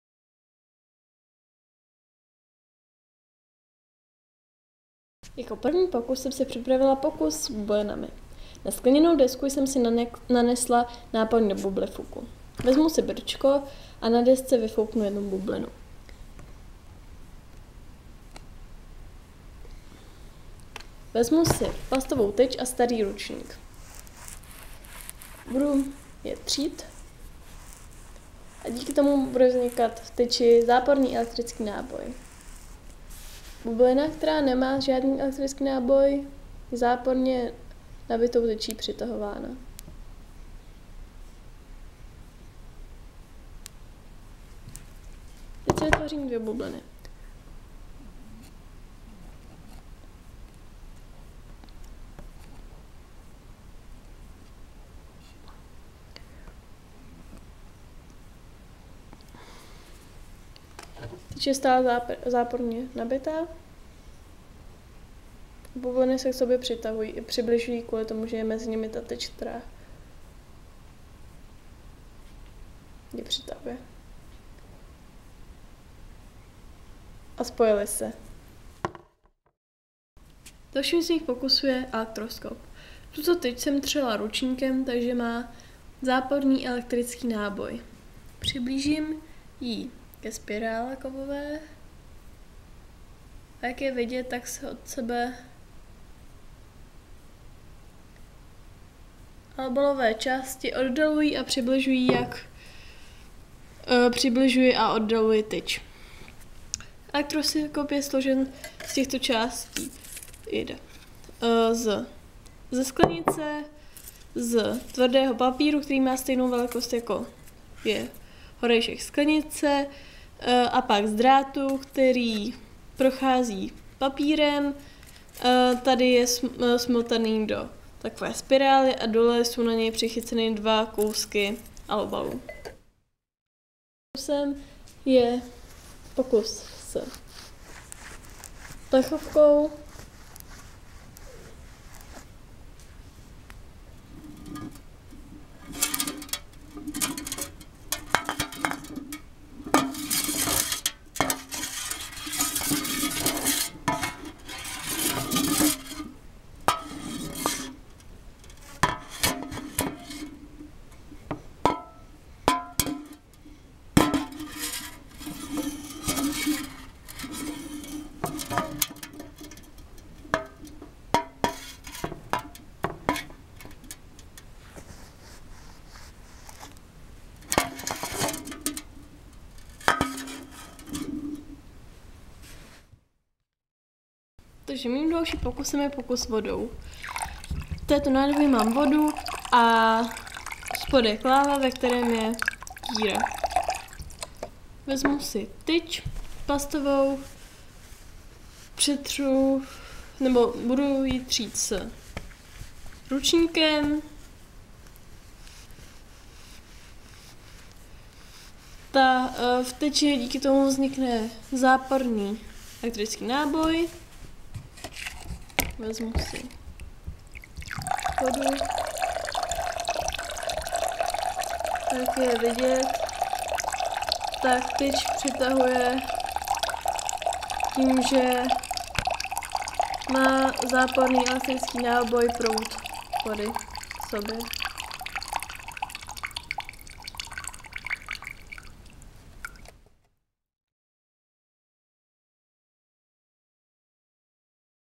FYZ | Elektrostatika
Několik pokusů z elektrostatiky: působení elektrického pole na tělesa kolem nás. Videa vznikla v rámci distanční výuky fyziky v tercii školního roku 2020/21.